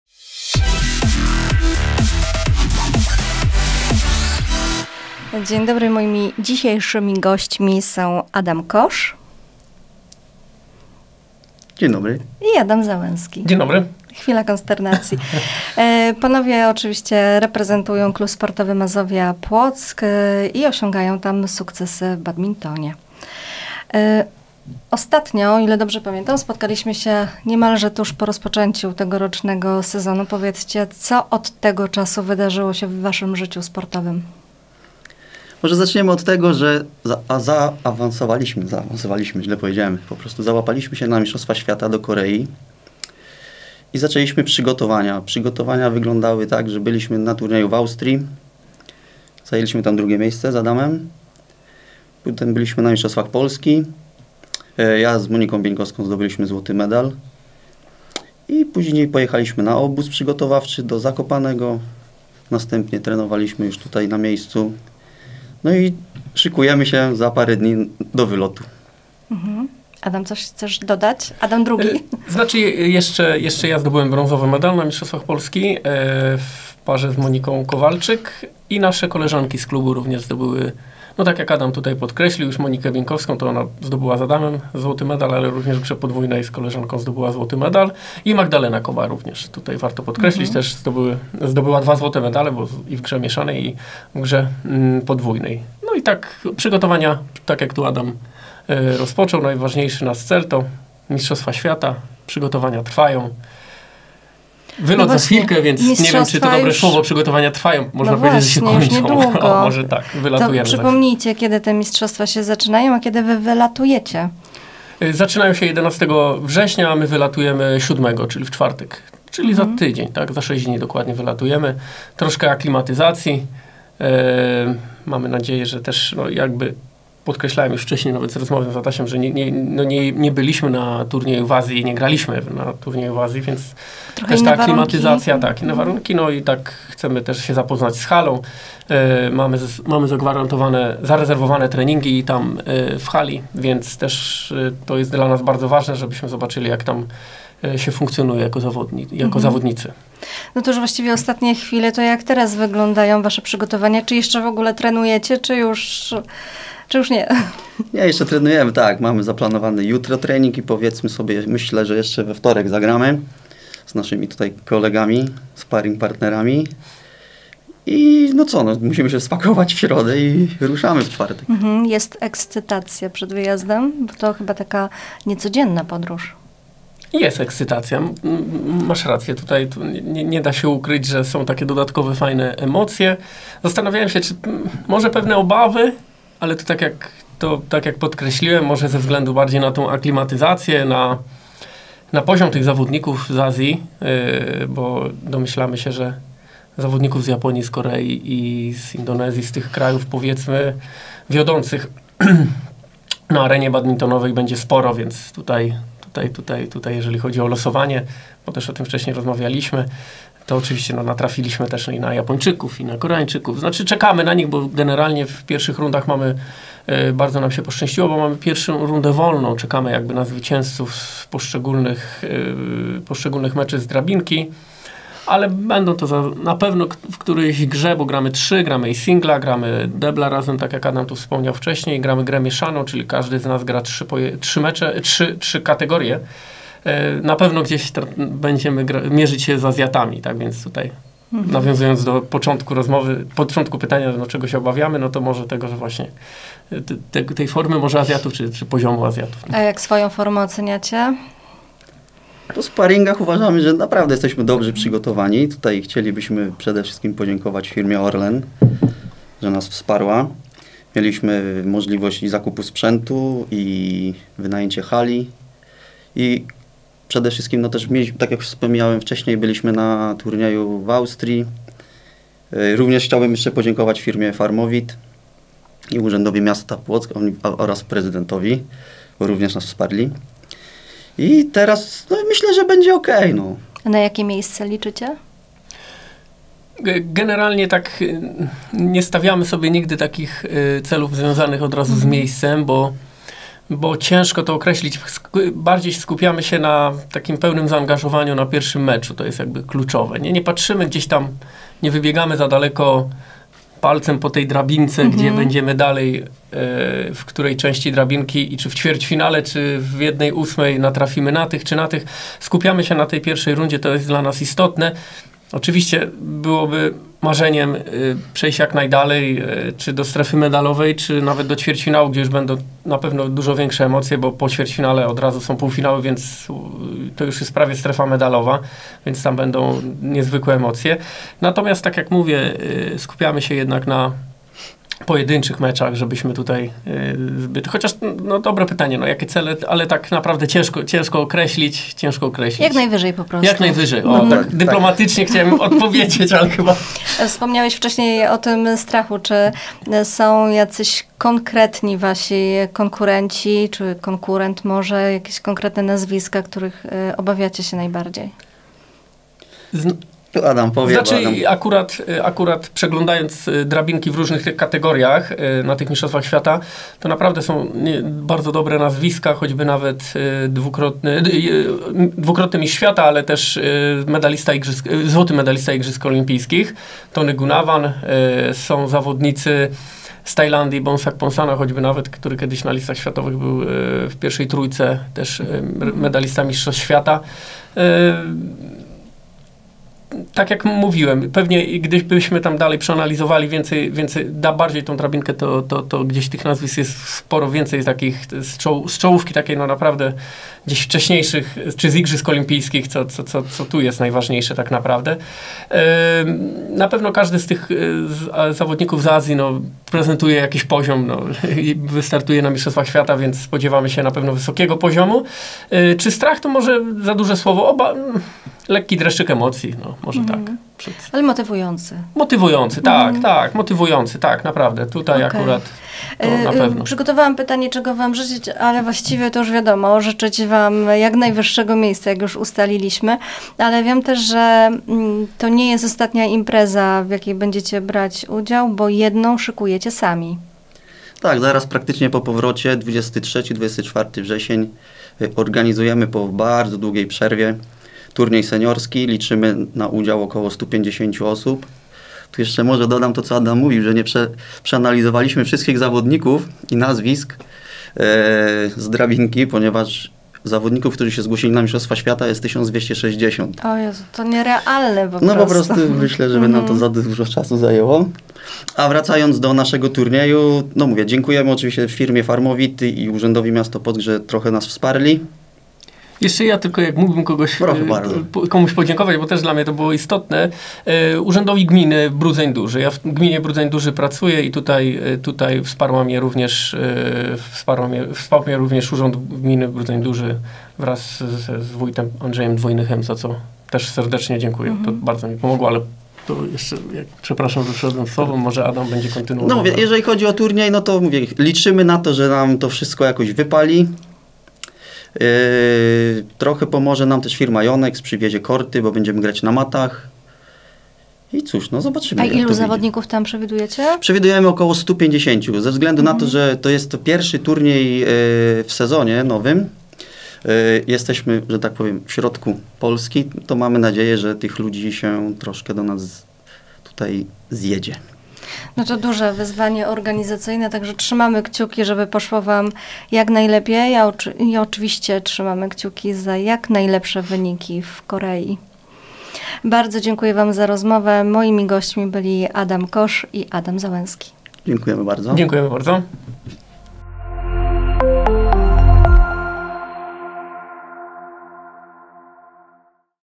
Aktywnie na Sportowo #13 - Badminton KS Masovia Płock
Jak mija im tegoroczny sezon? W jaki sposób przygotowują się do mistrzostw w Korei? I co planują w Płocku po powrocie? O tym rozmawiamy z Adamem Koszem i Adamem Załęskim z Klubu Sportowego Masovia Płock.